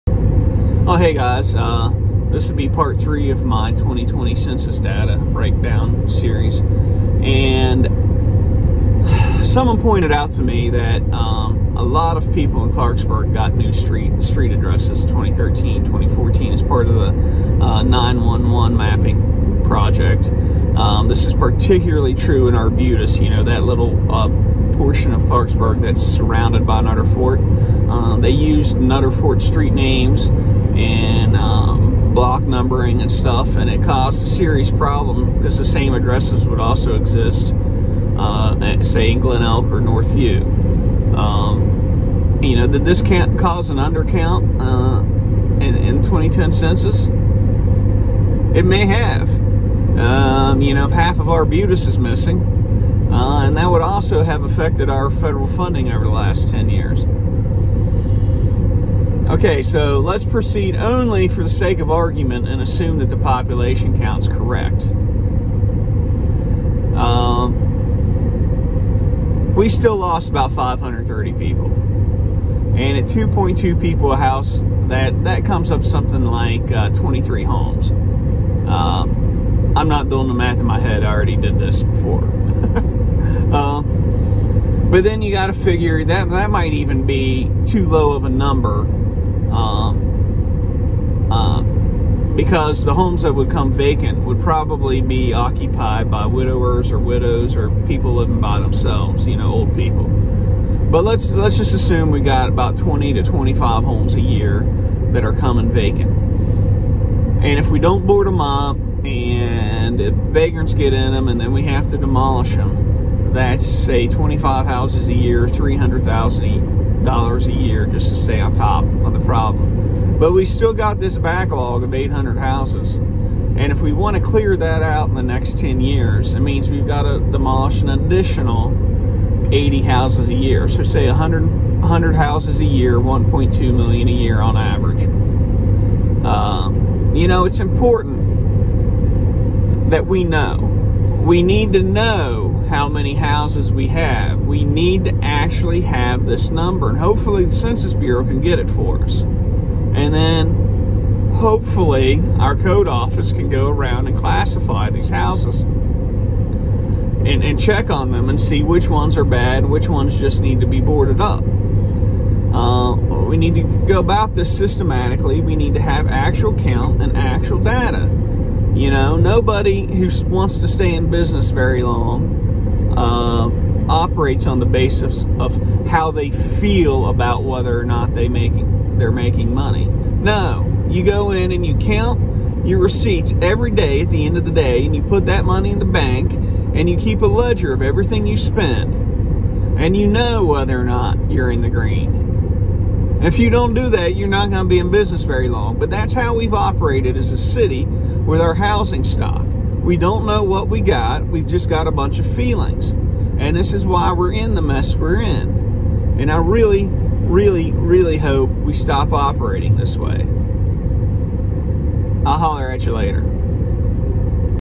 2020 Census Breakdown, Part III
To clear our vacant housing backlog we would need to demolish 100 houses a year at a cost of about $1,200,000 a year.